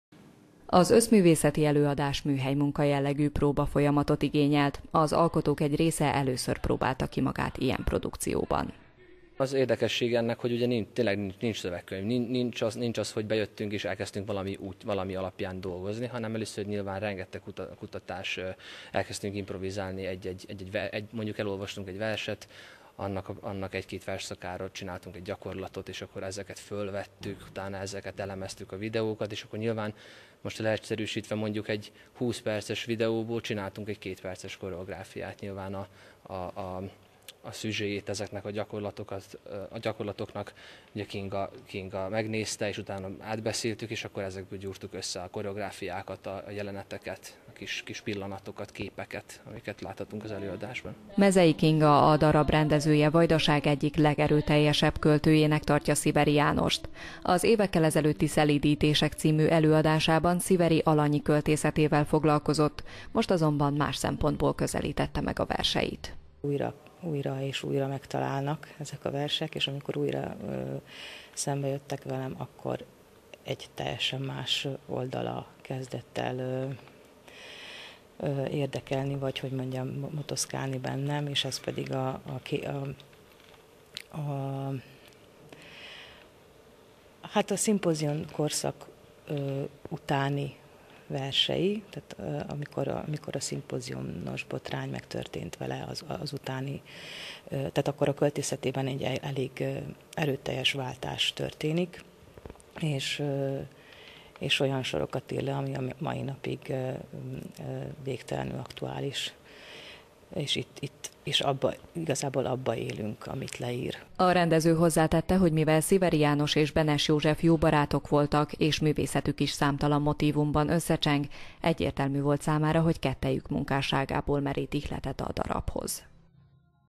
A Hidegpróba a Zentai Magyar Kamaraszínházban
A Zentai Magyar Kamaraszínház és a Novem Színházi Szervezet pénteken tartotta meg a 2023/2024-es évad első bemutatóját.